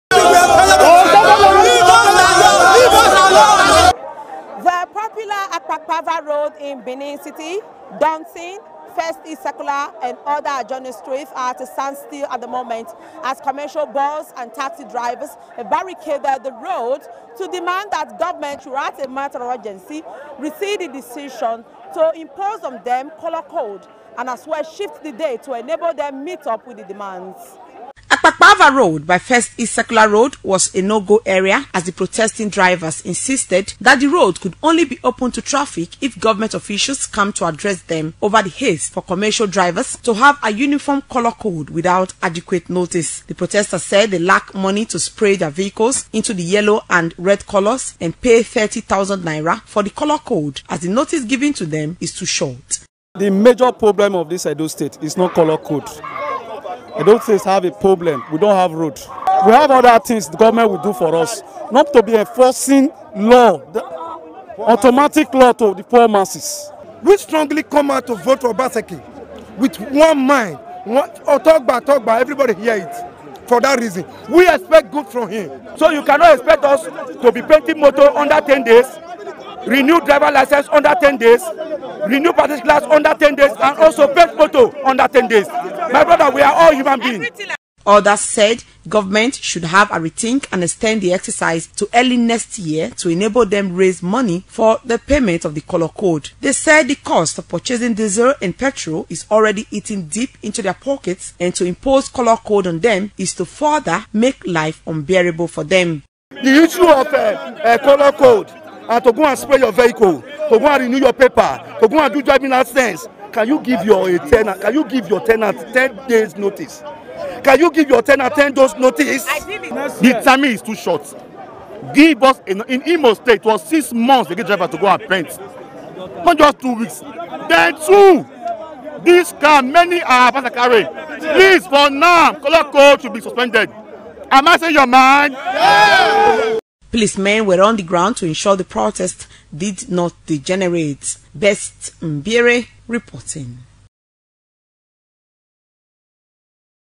Commercial Driver In Benin Protest Against Enforcement Of Colour Code By Edo State Govt.
There was confusion in parts of Benin City today, following protest by commercial bus and taxi drivers over the enforcement of uniform colour code by the Edo State Government.

Best Mbiere reports that the drivers blocked major roads, a situation that left commuters and other road users stranded for hours.